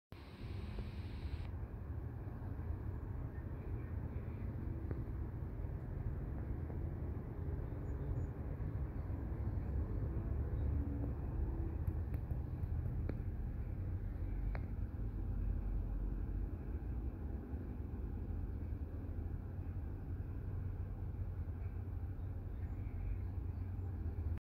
Alligator Lizard eats Mealworm 2
This wild alligator lizard came to visit. It was totally unafraid and seemed hungry so I tossed it a worm. Part 2 is after eating the worm and it licks lips, sniffs around and looks clever.